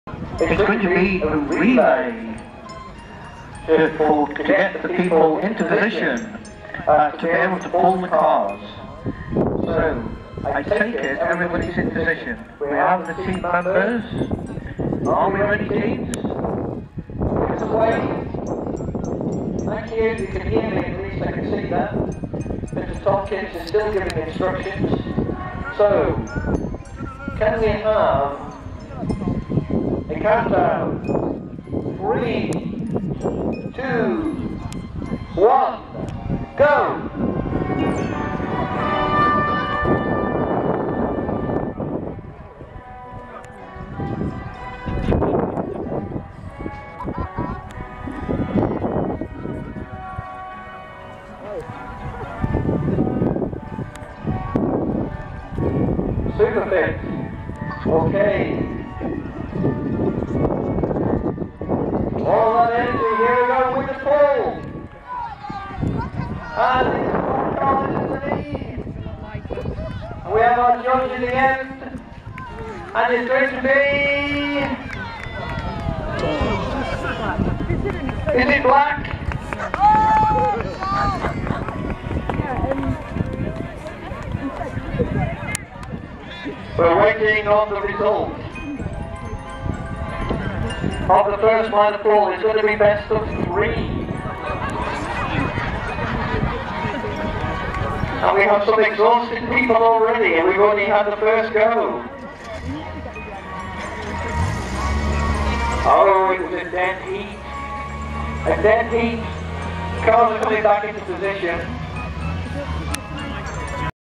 Morris Minor Owners Club National Rally 2016 Minor pull (1)
The first attempt at the "Minor pull" competition by the Young Members Branch of the MMOC. This was at the National Rally and MMOC 40th birthday event at Catton Hall in Derbyshire on Sunday, 3.7.2016.